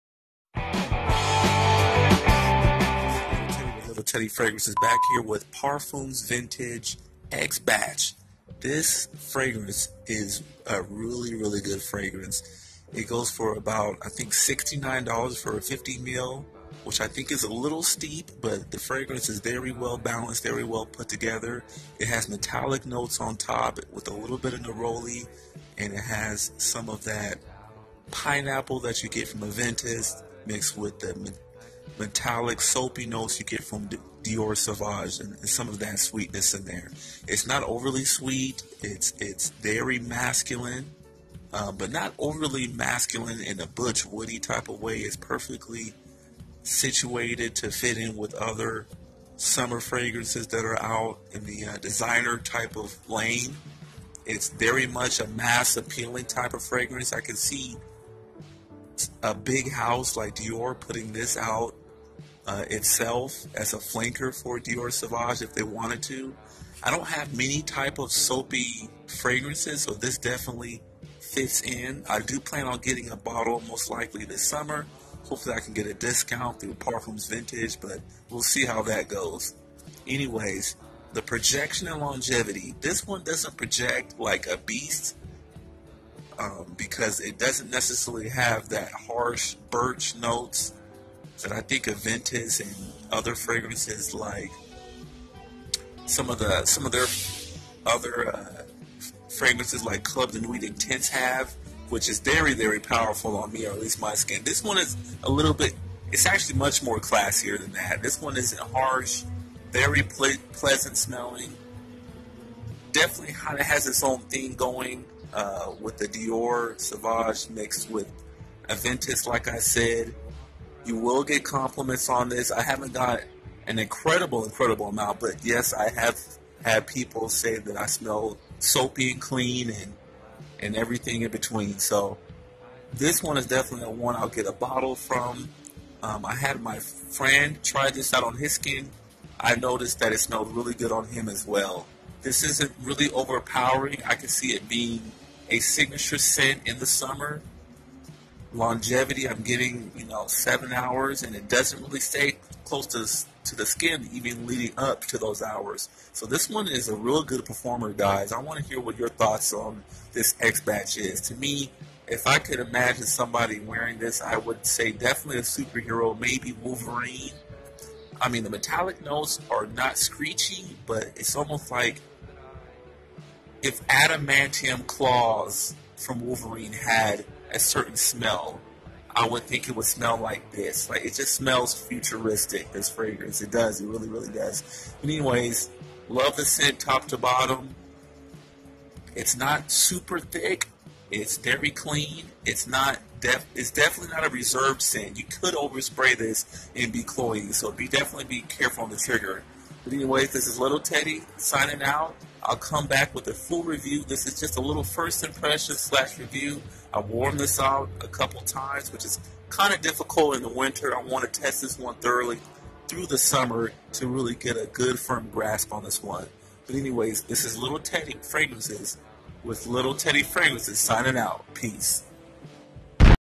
Parfums Vintage - X Batch Mini Fragrance Review
Great spring summer scent. Metallic in a good way and masculine.